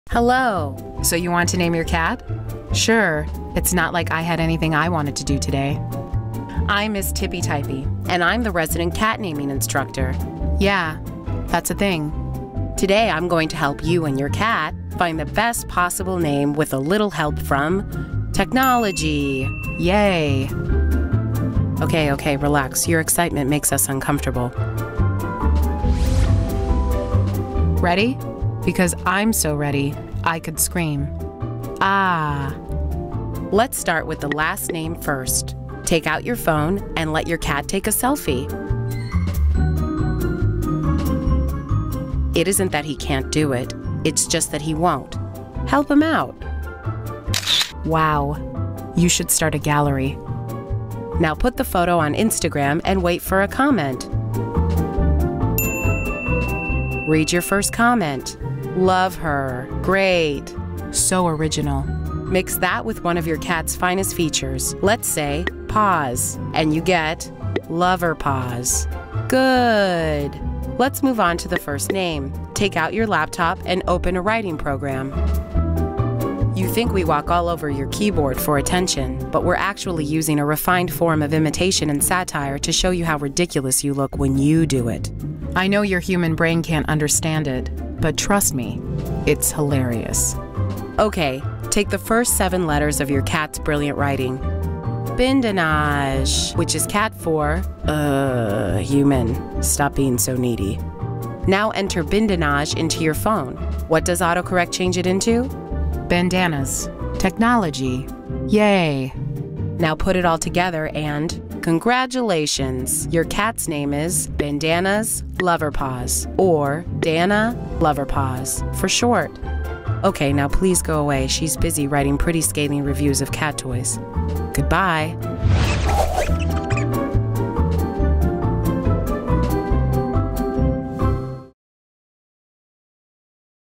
How to Name Your Cat Using Technology
Technology has enabled us to do many paw-some things, but did you know it can help you name your cat?! Don't believe us? Enjoy this tutorial, we think you'll love the results!

Let's face it: cats are glorious creatures and they deserve names that fit them. We know choosing the perfect name for your cat can be difficult so Friskies is here to help!